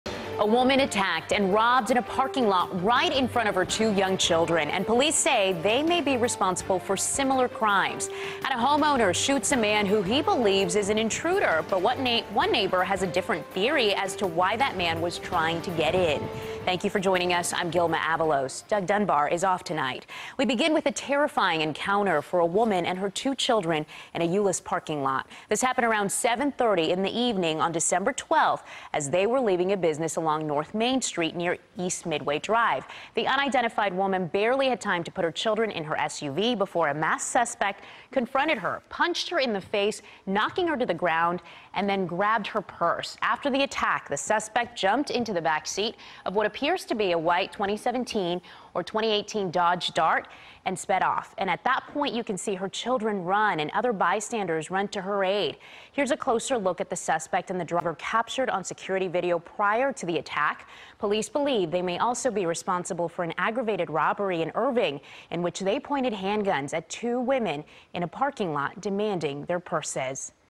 Mother Out With Her Kids Attacked, Robbed In Euless Parking Lot
A masked suspect confronted the woman, punched her in the face knocking her to the ground and grabbed her purse.